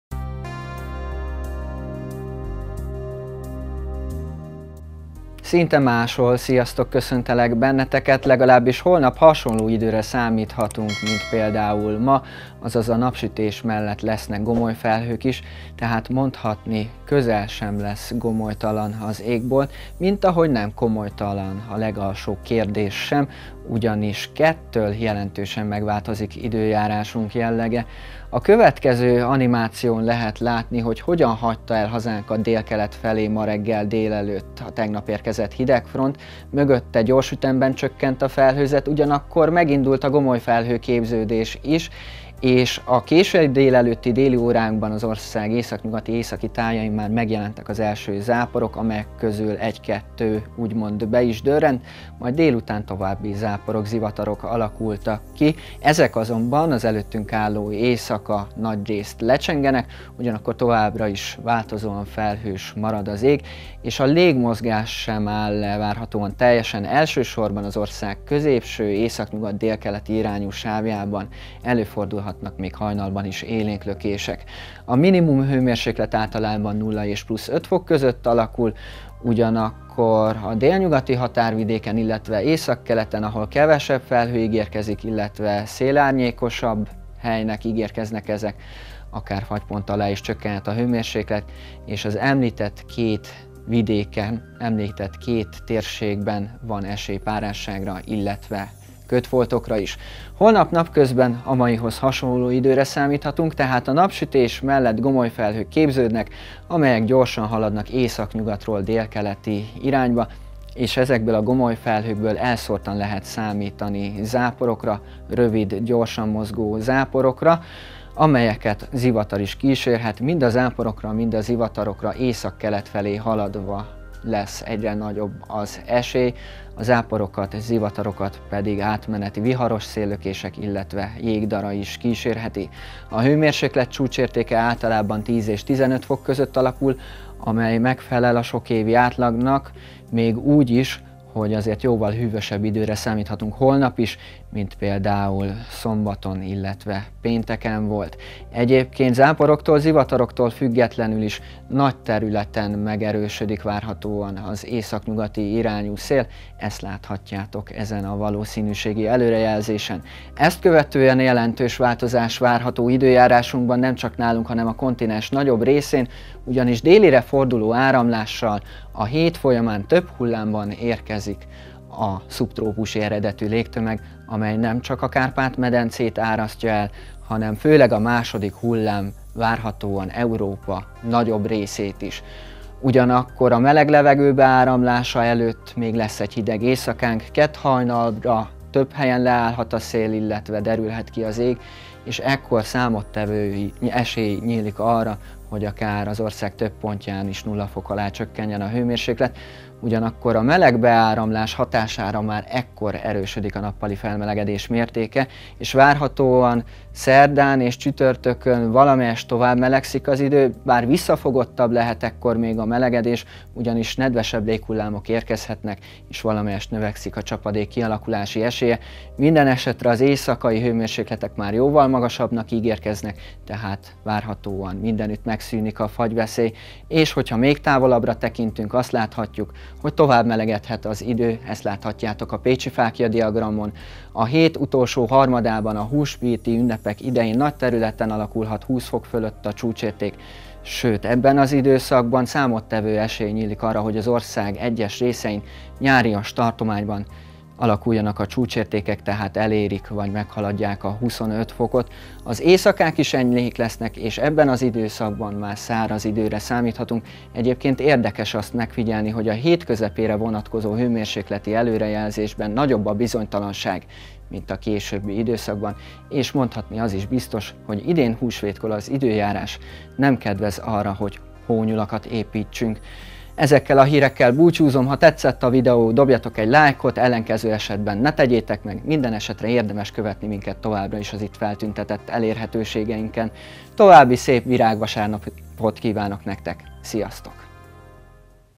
Nem gomolytalan
Hétfőn is a vasárnapihoz hasonló időre számíthatunk: a napsütés mellett képződő gomolyfelhőkből elszórtan alakulhatnak ki záporok, sőt akár zivatarok is. A hideg kedd reggelt követően gyökeres változás várható: délire forduló áramlással jelentős melegedés kezdődik.

Ha érdekel az időjárás, akkor kövess be minket az HungaroMet közösségi média felületein: